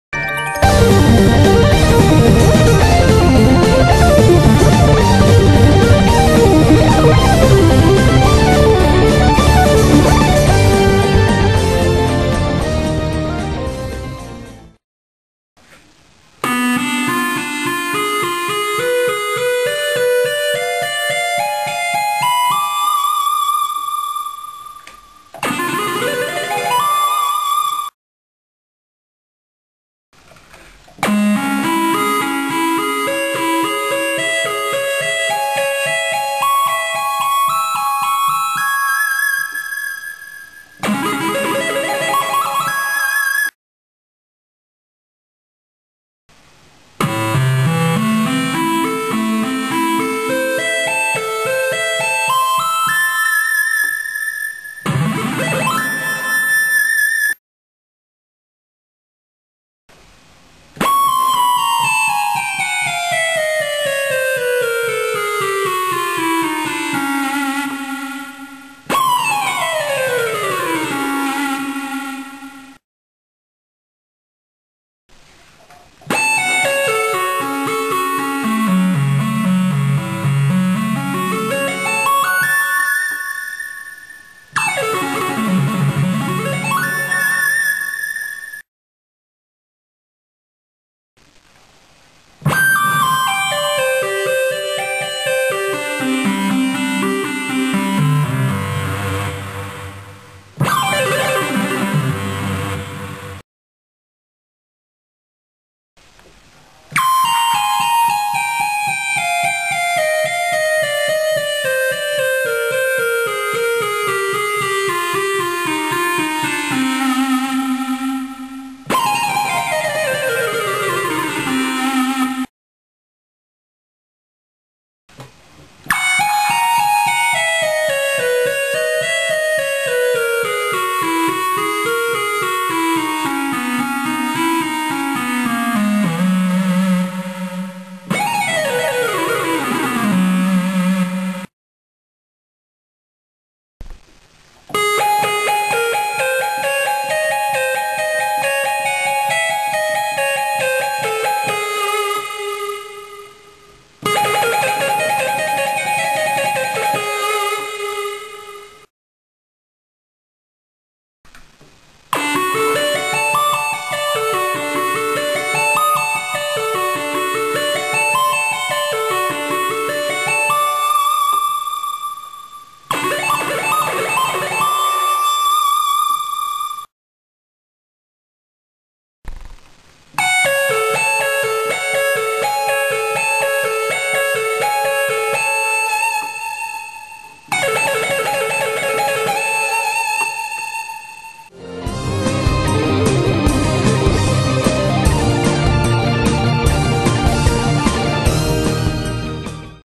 Shred Keyboard Licks I - Miguel Garcia
Here are 11 simple, typical and useful shred keyboard licks played slow and fast for you to learn and use!